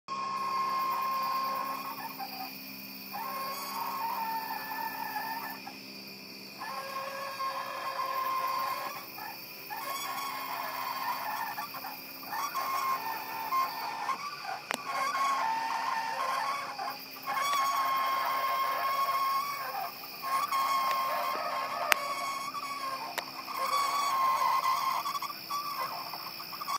G scale UP caboose dragging the brakes
G scale freight dragging the brakes on the caboose.